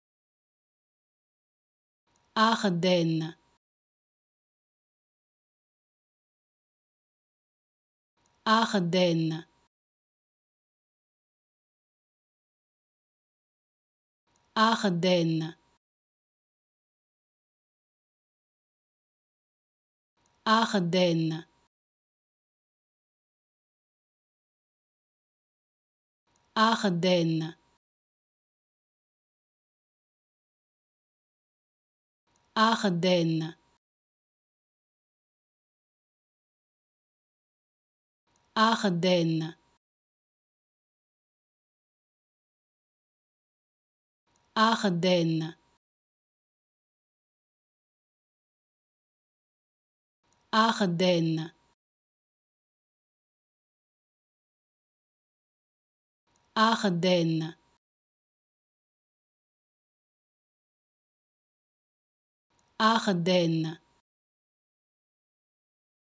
How to Pronounce ''Ardennes'' Correctly in French
Learn how to say and properly pronounce ''Ardennes'' in French with this free pronunciation tutorial. Apprenez à prononcer des mots en français grâce à nos tutoriels.